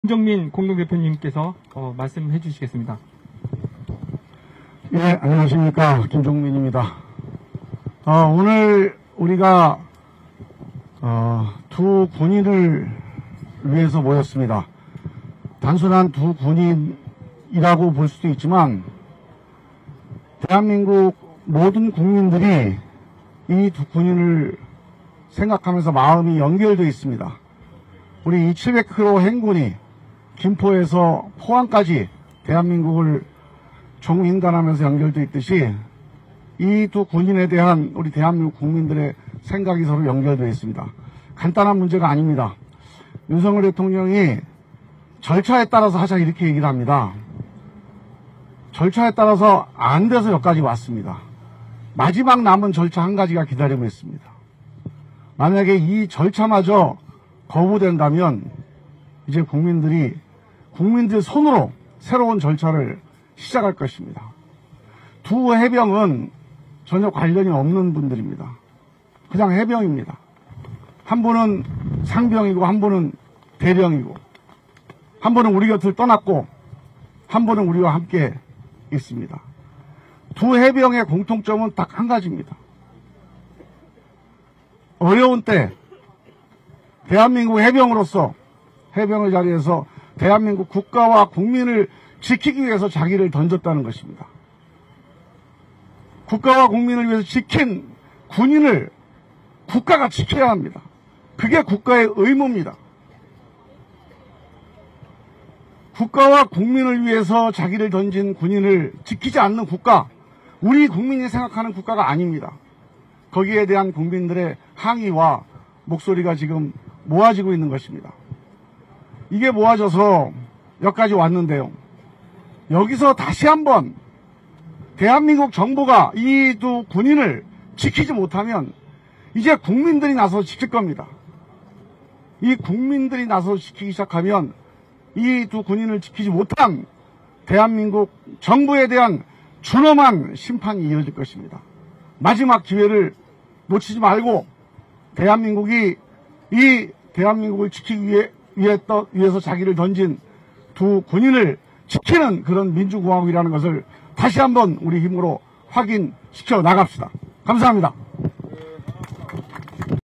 국가가 국민 지키지 않으면 국민이 나서야, 준엄한 심판 뿐! - 김종민 새로운미래 의원 / ⌜생명・정의・자유를 위한 해병대 700km 연대의 행군⌟ 4차 행군
⌜생명・정의・자유를 위한 해병대 700km 연대의 행군⌟ 4차 행군 현장을 담았습니다.